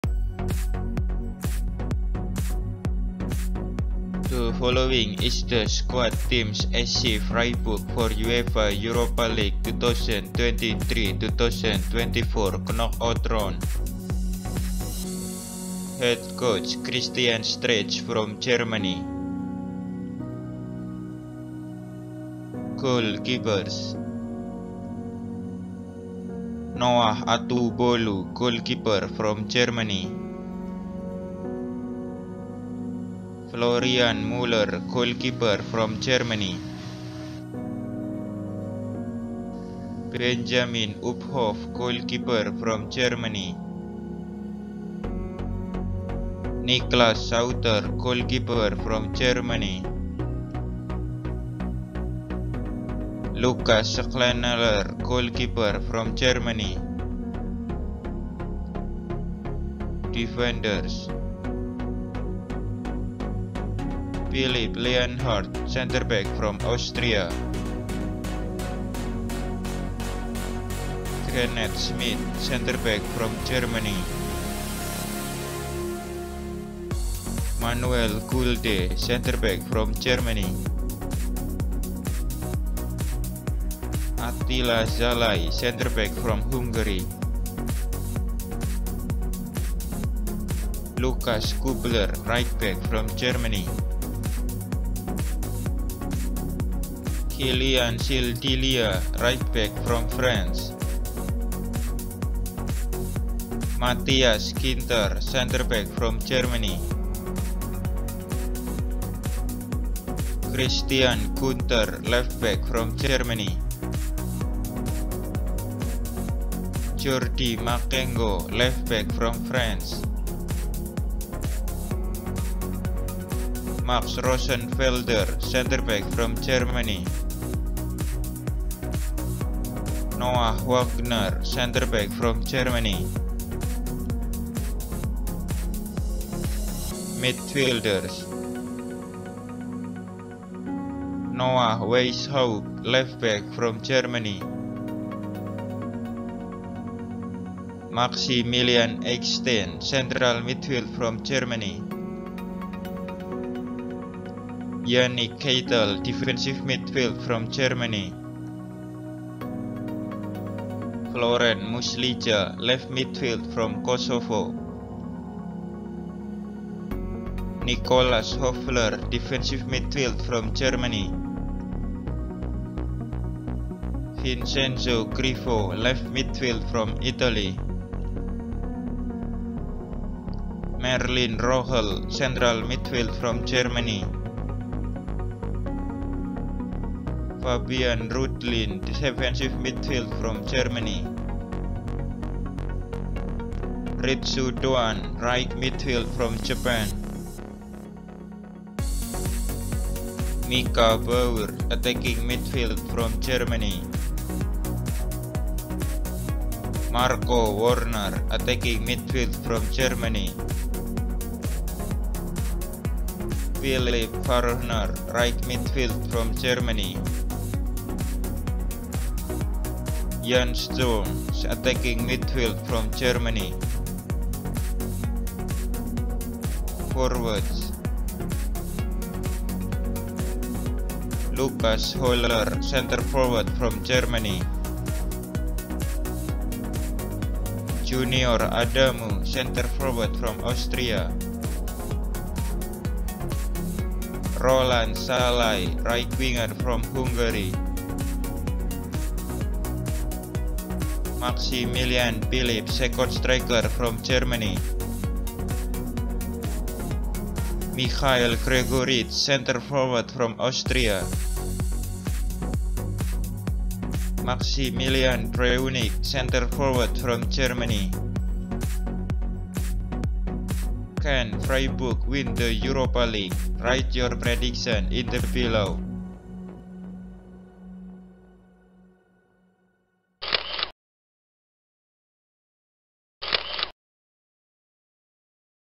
[New Players 🆕] SC FREIBURG 🇩🇪 MEN SQUAD for UEFA Europa League 2023/2024 Knock-out Round
Hello Guys, This video is Squad SC Freiburg  for UEFA Europa League 2023/2024 Knock-out Round

Head Coach :
Christian Streich (Germany)

Goalkeepers :
Noah Atubolu (Germany)
Florian Muller (Germany)
Benjamin Uphoff (Germany)
Niklas Sauter (Germany)
Lukas Schneller (Germany)

Defenders :
Philipp Lienhart (Austria)
Kenneth Schmidt (Germany)
Manuel Gulde (Germany)
Attila Szalai (Hungary)
Lukas Kubler (Germany)
Kiliann Sildilla (France)
Matthias Ginter (Germany)
Christian Gunter (Germany)
Jordy Makengo (France)
Max Rosenfelder (Germany)
Noah Wagner (Germany)

Midfielders :
Noah Weißhaupt (Germany)
Maximilian Eggstein (Germany)
Yannik Keitel (Germany)
Florent Muslija (Kosovo)
Nicolas Hofler (Germany)
Vincenzo Grifo (Italy)
Merlin Rohl (Germany)
Fabian Rudlin (Germany)
Ritsu Doan (Japan)
Mika Baur (Germany)
Marco Worner (Germany)
Philip Fahrner (Germany)
Yann Sturm (Germany)

Forwards :
Lucas Holer (Germany)
Junior Adamu (Austria)
Roland Sallai (Hungary)
Maximilian Philipp (Germany)
Michael Gregoritsch (Austria)
Maximilian Breunig (Germany)